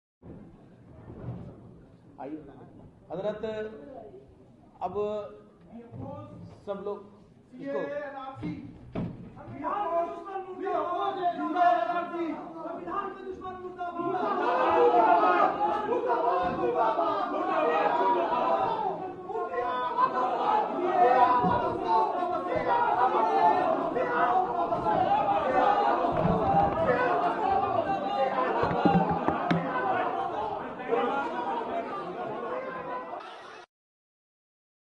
सीएए वरून आरएसएस'च्या उलेमा परिषदेत हाणामारी
नवी दिल्ली: राष्ट्रीय स्वंयसेवक संघाने बोलावलेल्या राष्ट्रीय उलेमा परिषदेत आज राडा झाला. नागरिकत्व सुधारणा कायद्याच्या समर्थनासाठी हि परिषद बालवण्यात आली होती. आरएसएसचे नेते इंद्रेश कुमार समोरच मुस्लिमांच्या दोन गटांमध्ये वाद झाला आणि प्रकरण हाणामारीपर्यंत गेले.